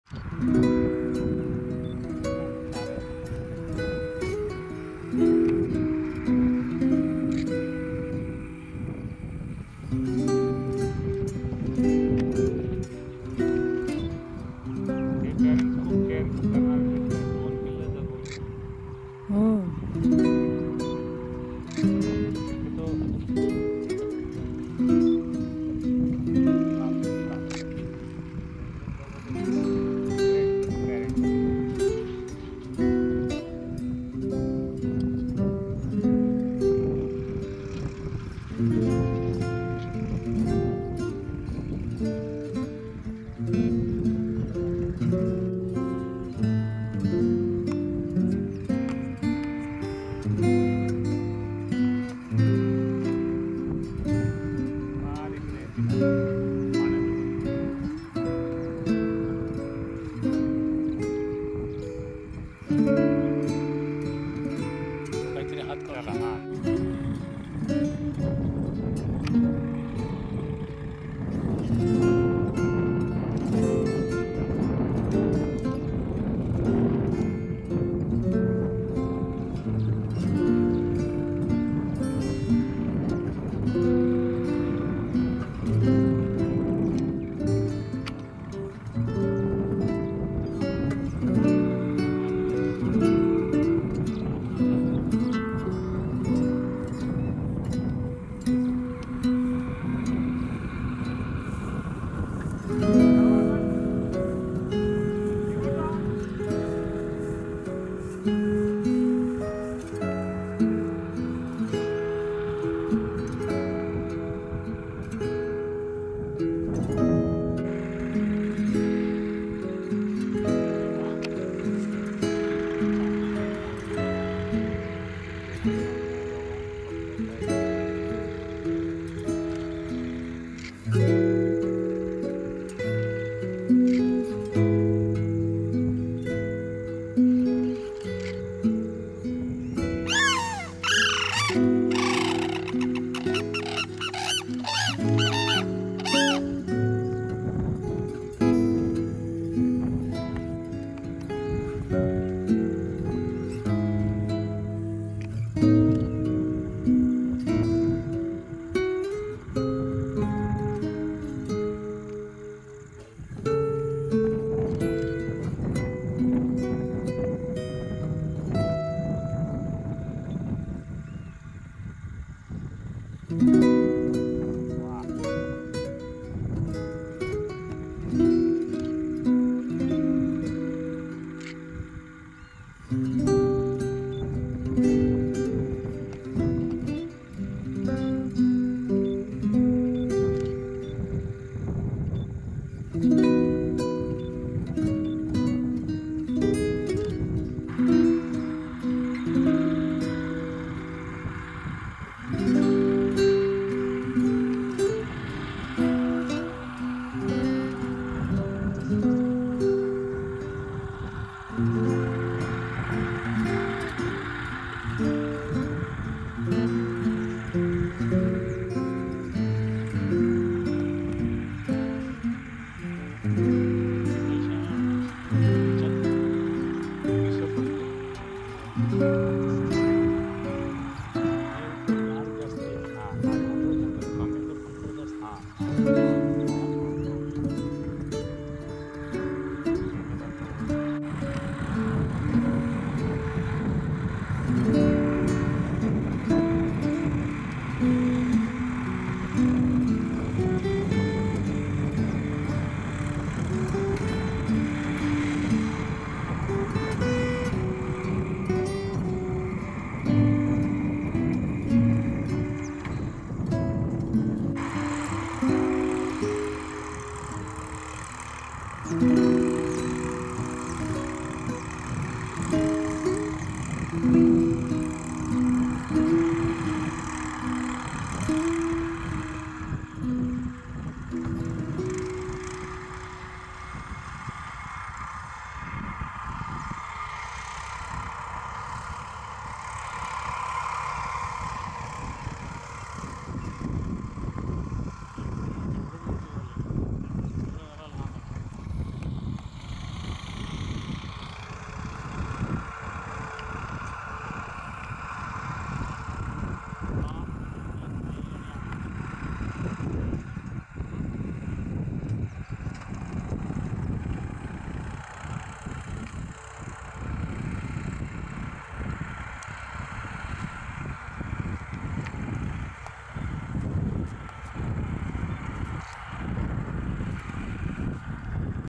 SARUS CRANES
HELLO DEAR FRIENDS. , SHARING WITH YOU A VIDEO OF A PAIR OF SARUS CRANES WITH TWO BABIES . IN OUR RECENT VISIT TO NALSAROVAR, GUJRAT, INDIA, WE GOT A CHANCE TO SEE THIS FAMILY. THIS BIRD LIVES IN AGRICULTURAL LANDS IN CLOSE PROXIMITY TO HUMANS.